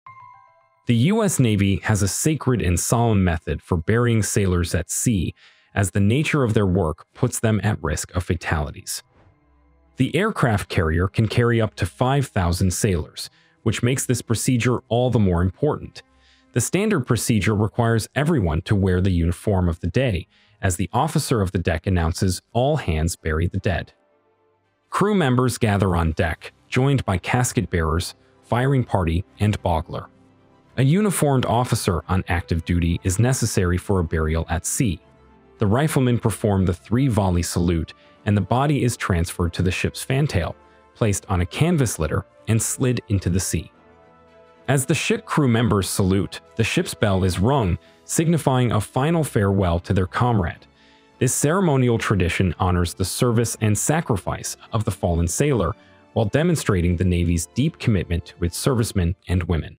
This is how the US Navy buries sailors in the middle of the sea
Discover the incredible technique used by the US Navy to bury sailors at sea. This centuries-old tradition involves a unique ritual that honors fallen servicemen and women. By following strict protocols, the Navy ensures that the burial is conducted with dignity and respect.